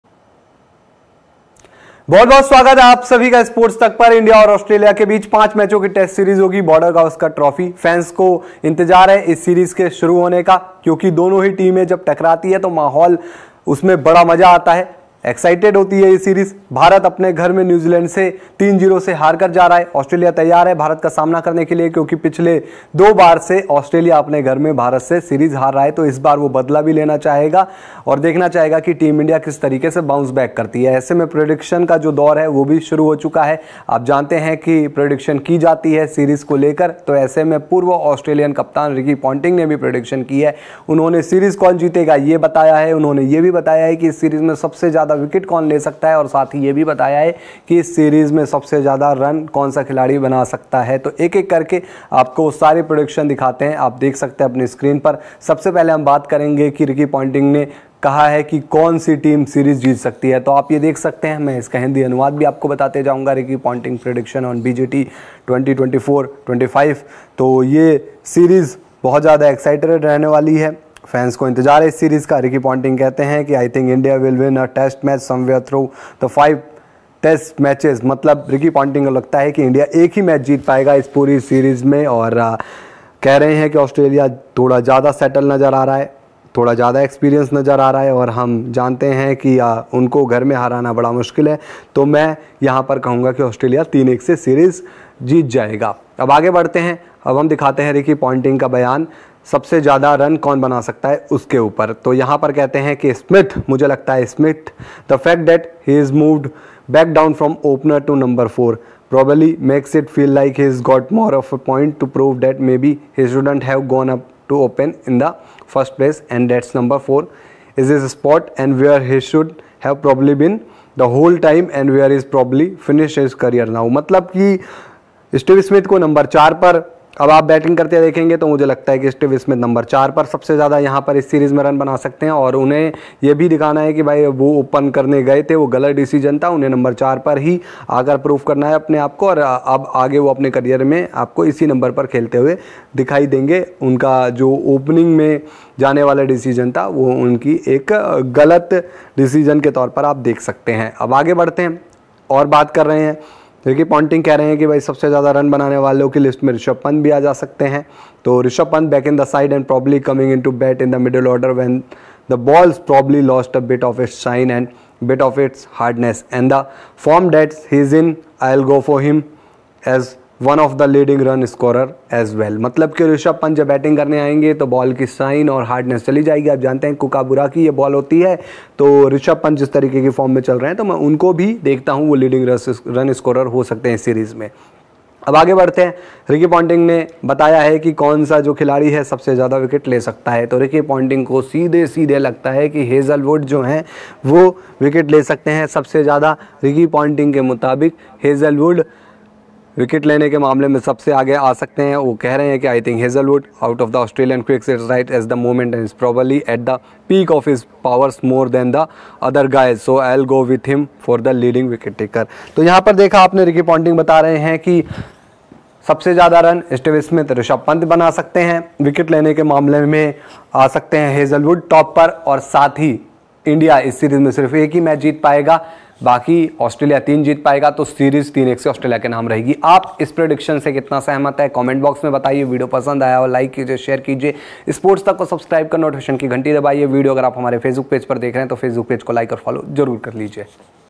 IND-AUS में कौन बनेगा Champion, किसके होंगे सबसे ज्यादा रन और विकेट, Ponting ने की बड़ी भविष्यवाणी
Try Fantasy Yatra, India ka Naya fantasy -

Former Australian captain Ricky Ponting has made his predictions for the upcoming five-match Test series between India and Australia. The 2024-25 edition of the Border-Gavaskar Trophy is set to start on November 22 in Perth, and the next four matches are scheduled to take place in Adelaide, Brisbane, Melbourne, and Sydney, respectively.

Vikrant Gupta
Sports Tak
Aaj Tak
Aaj Taj Live
Sports Tak